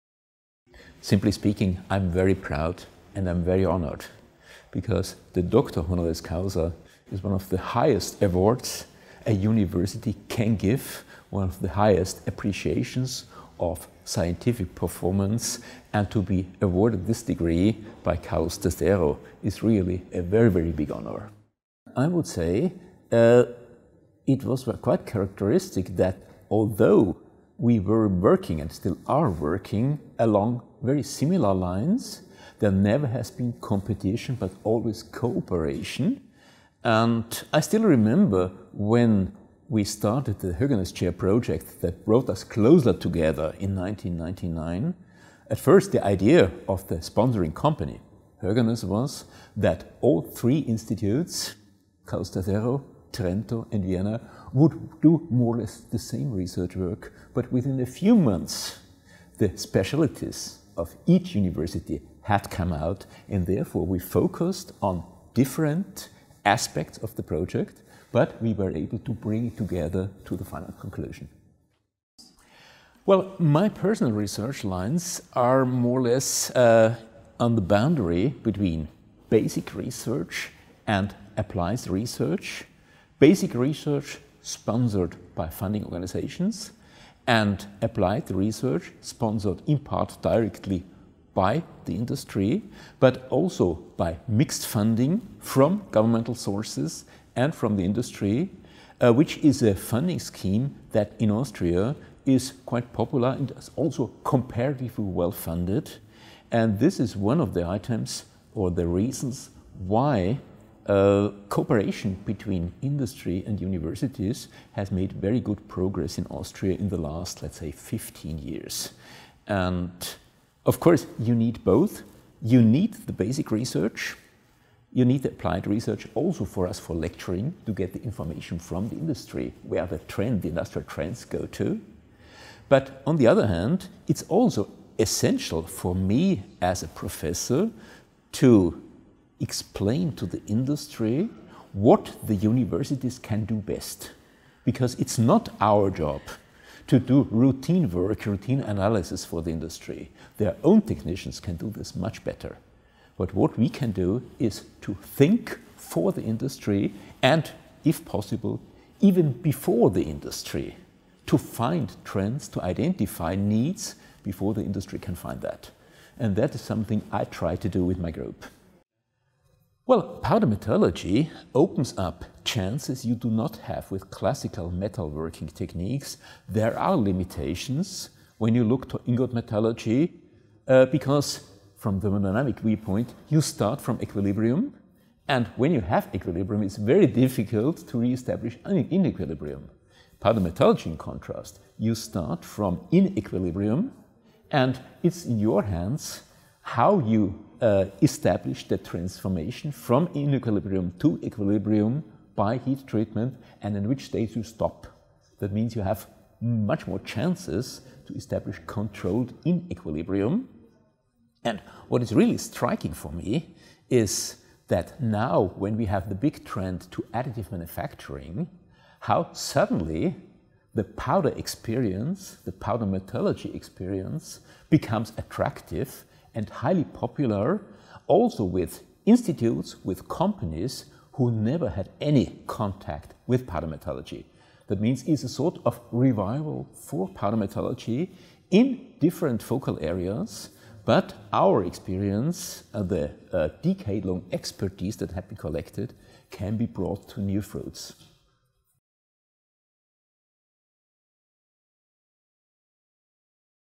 Entrevista a Herbert Danninger
Catedrático de la TU de Viena. Investido Doctor Honoris Causa por la Universidad Carlos III de Madrid, el viernes 29 de enero de 2016, por el rector de la UC3M, Juan Romo, por sus méritos como investigador en el ámbito de la Ciencia e Ingeniería de los Materiales.